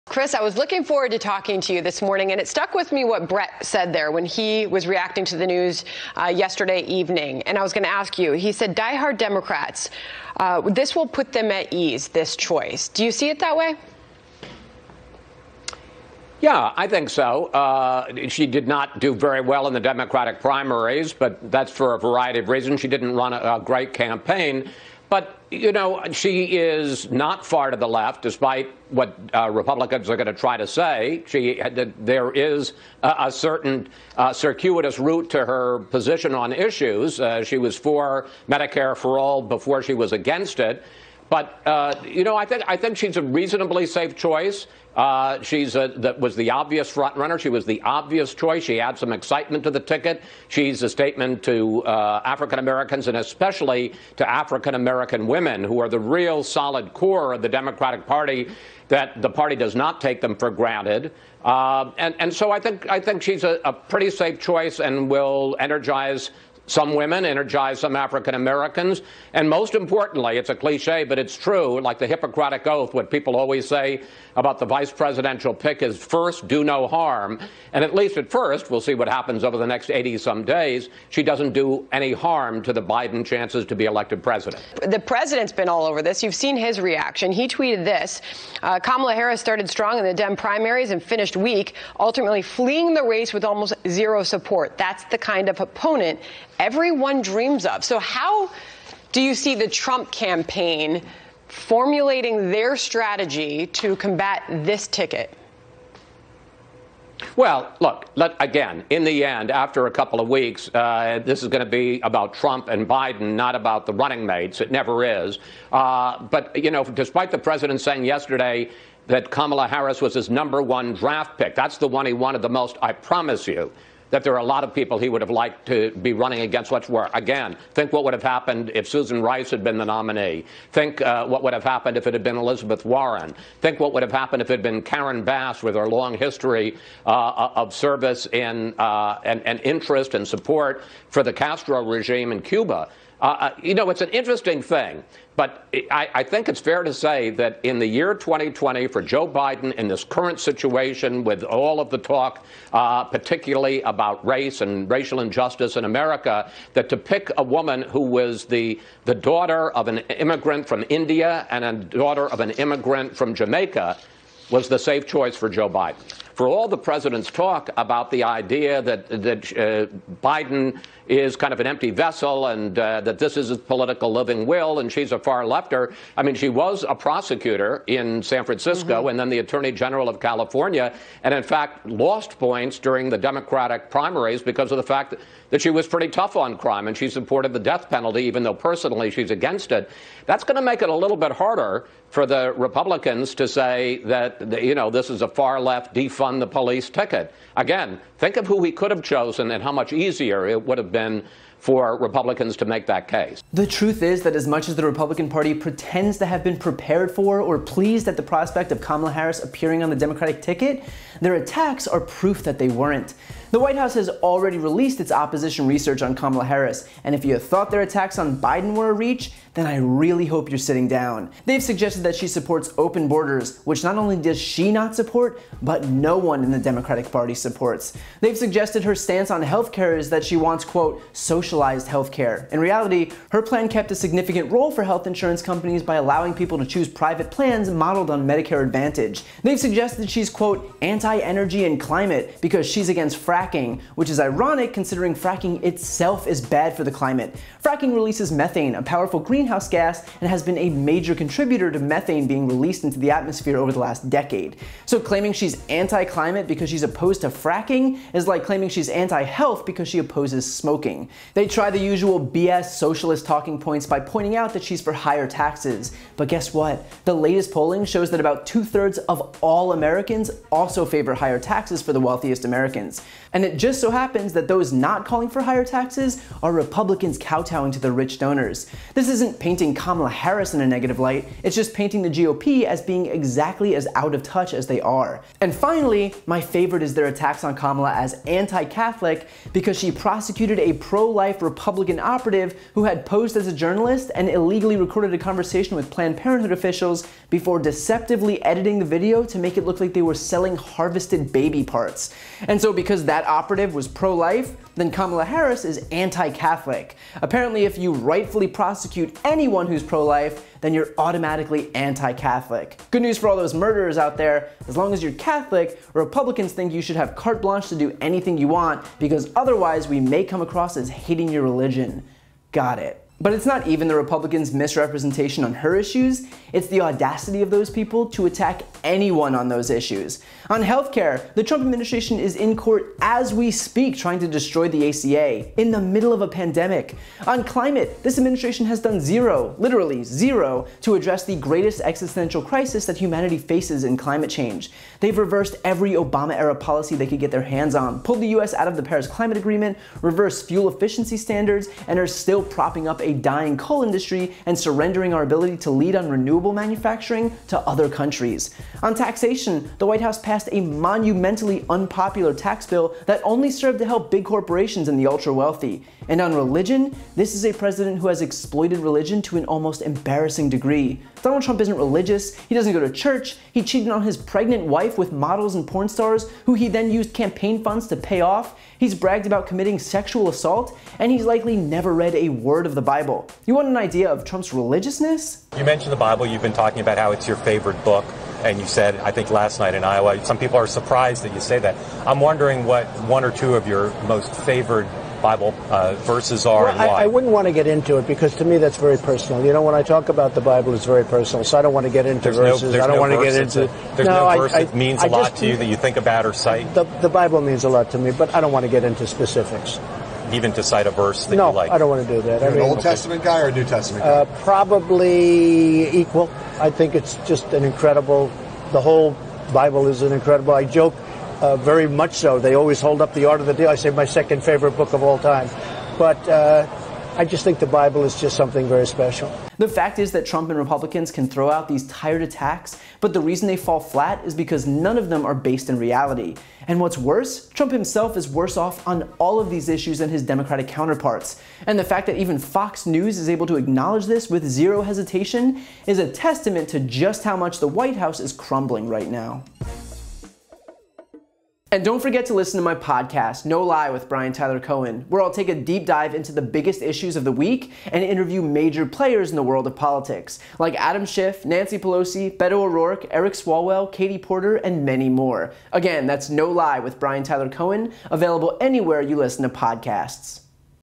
Chris Wallace debunks Trump's attacks on Kamala Harris in blow to Republicans
BREAKING: Fox News' Chris Wallace just debunked Trump's attacks against Kamala Harris in major blow to Republicans.

To tell Congress not to settle for Trump's illegal sham executive order, sign

(or search for "No Lie with Brian Tyler Cohen" on your preferred podcast platform)